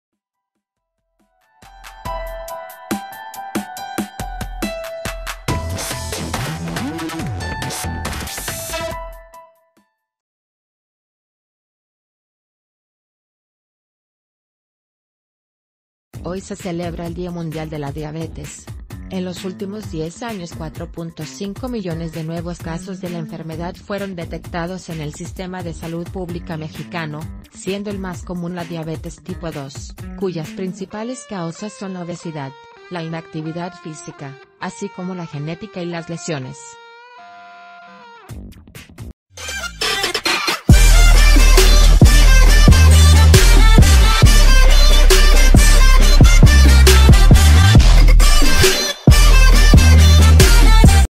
Diabetes: Dulce enemigo del mexicano
Hoy se celebra el día mundial de la diabetes. En los últimos 10 años 4.5 millones de nuevos casos de la enfermedad fueron detectados en el sistema de salud pública mexicano, siendo el más común la diabetes tipo II, cuyas principales causas son la obesidad, la inactividad física, así como la genética...